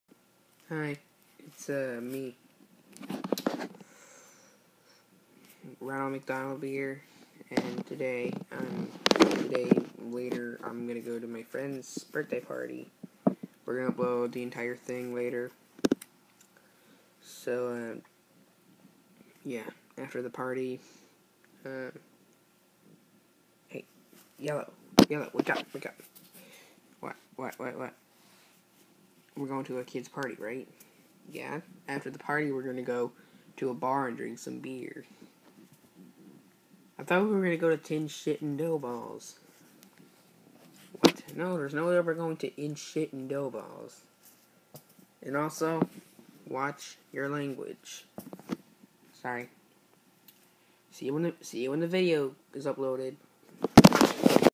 The Snuggle Bears Party Time Part 1
The Snuggle Bear have come back and are invited to Yoshislaver69s'
Birthday Party